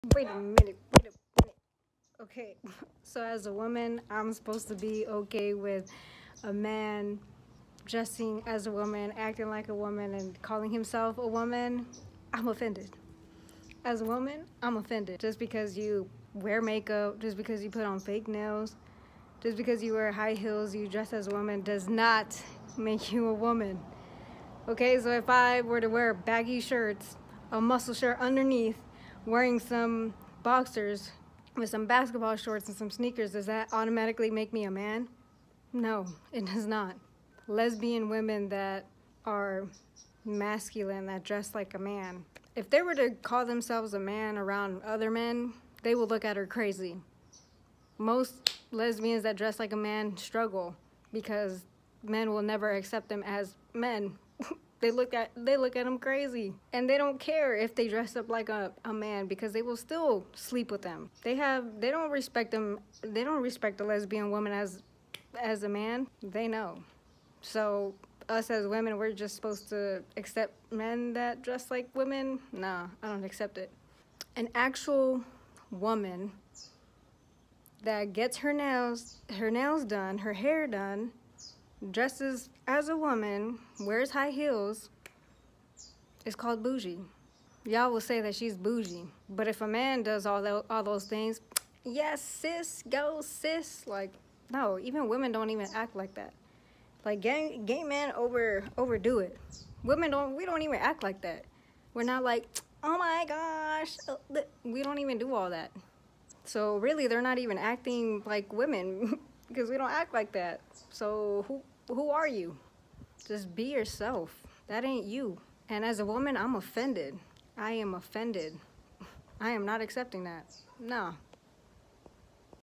As a woman I am offended!
Just because you dress like a woman doesn’t make you a woman. You don’t get a period every month, you don’t go through the same emotions as a woman, you can’t get pregnant like a woman. Period!! If you accept this lifestyle you are just as guilty I fear the Lord not man!